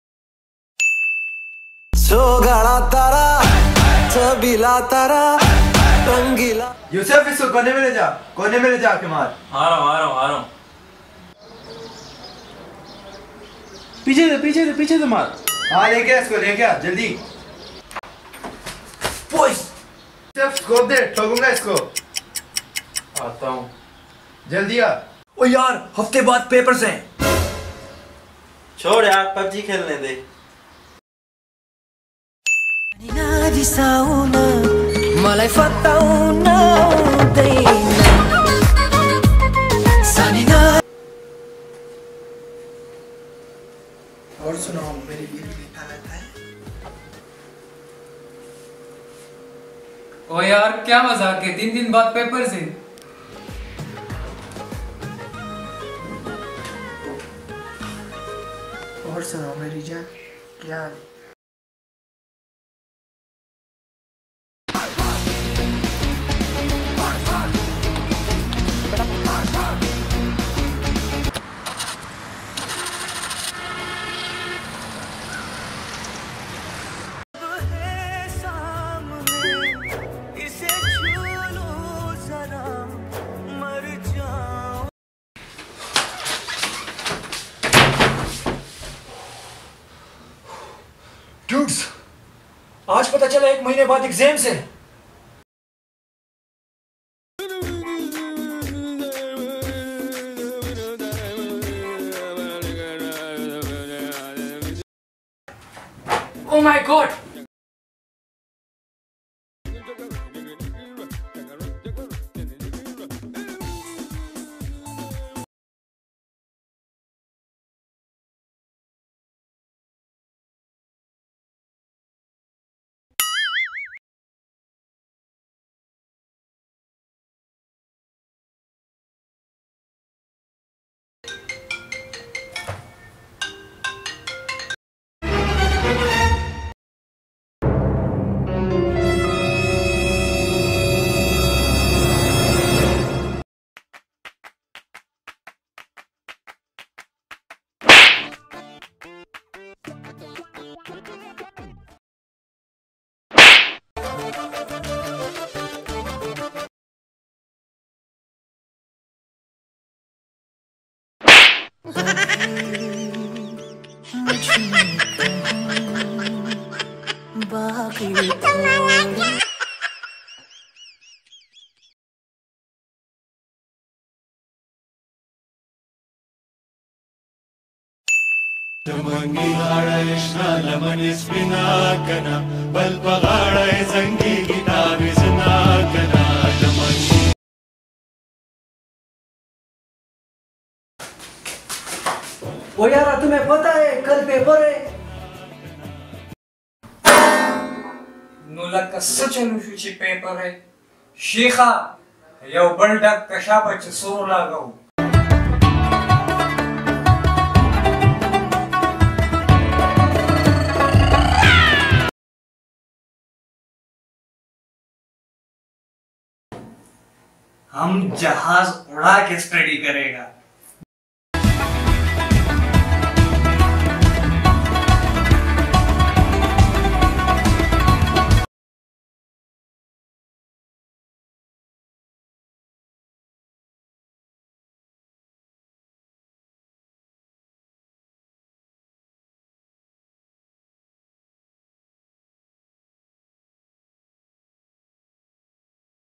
Hostel Life In Exams | Lewani vines | new video
Hostel life of students in Exams in different cities